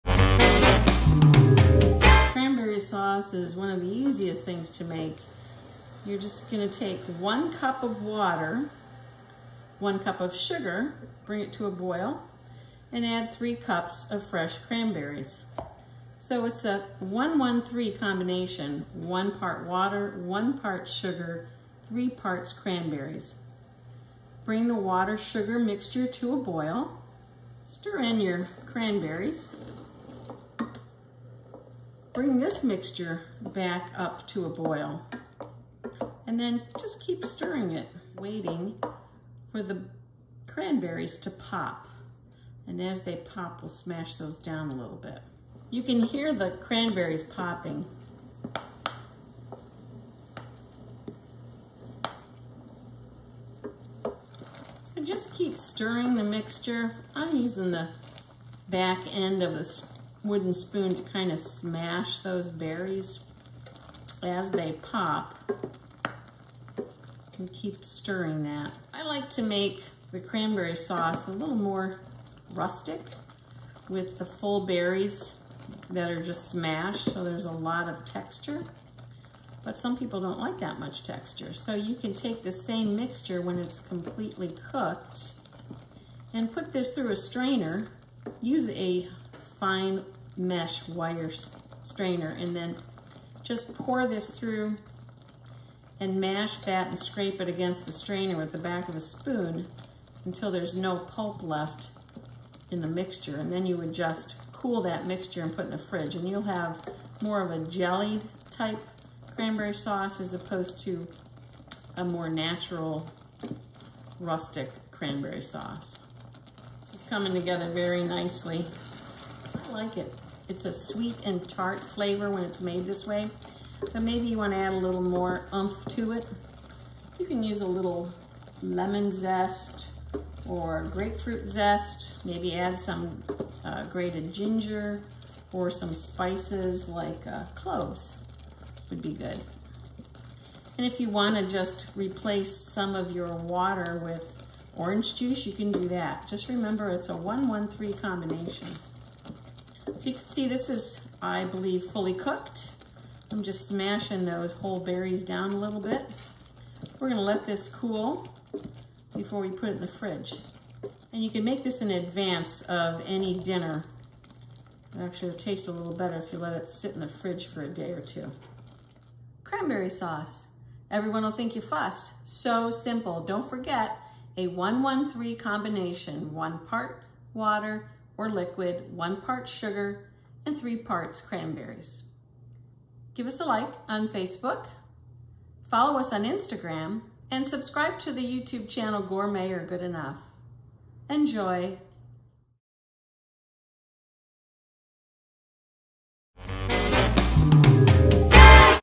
Discover the Quick and Simple Recipe for Easy Cranberry Sauce
This is the easiest Thanksgiving side dish to make ahead. Get your young chefs to help out with dinner prep by making this  super simple recipe.

Ingredients
1 cup water - could use a mixture of orange juice and water to equal 1 cup
1 cup sugar
3 cups fresh cranberries

Directions
Mix waiter and sugar in a 3 quart sauce pan.  Bring mixture to a boil.  Add cranberries and bring mixture back to a boil.  Lower temperature and stir occasionally until the berries start to pop.  Continue to stir, while using the back of a spoon to smash the berries as you stir.
Continue cooking until all the berries have popped.

Cool and refrigerate.

For additional flavor, add lemon or grapefruit zest or stir in some ginger or cloves.